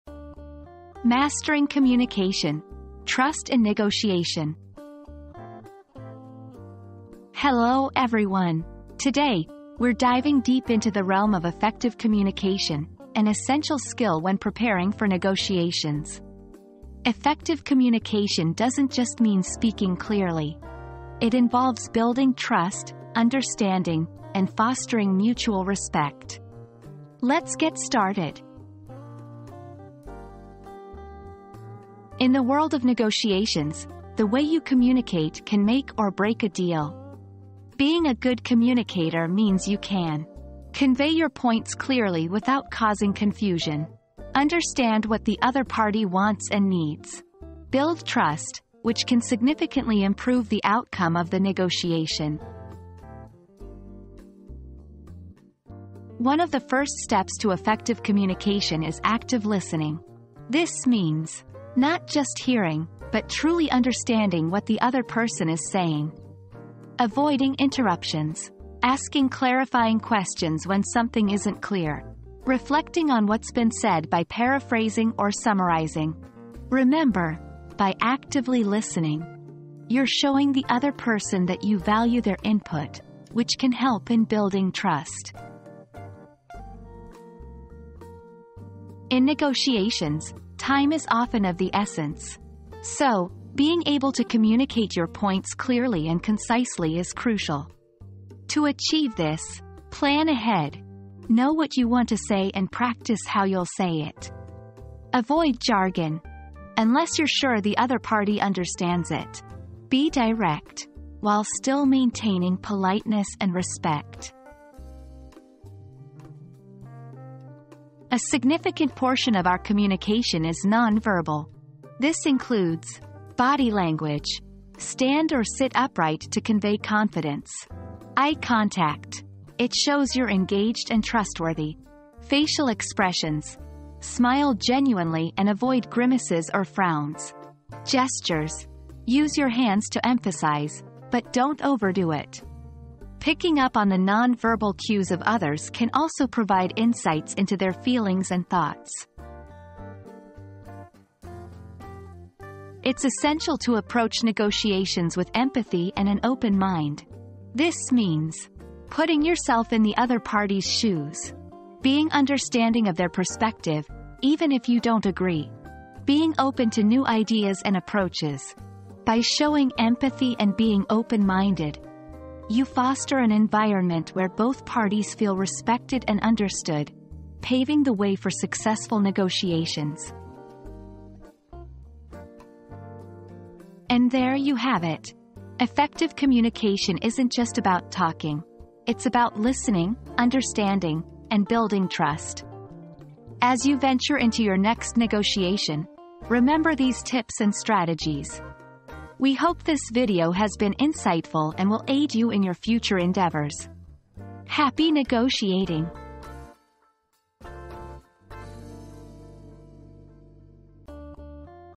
Mastering Communication: Trust in Negotiation
Mastering Communication: Trust in Negotiation • Learn the secrets to effective communication in negotiation and how it can build trust, understanding, and mutual respect. Discover the importance of active listening, clarity, and concise messaging. Enhance your non-verbal communication skills to achieve successful negotiations. Join us on this journey to master communication in negotiation!

00:00 • Introduction - Mastering Communication: Trust in Negotiation
00:31 • Why is Effective Communication Important?
00:55 • Active Listening
01:28 • Be Clear and Concise
01:57 • Non-Verbal Communication
02:32 • Empathy and Open-mindedness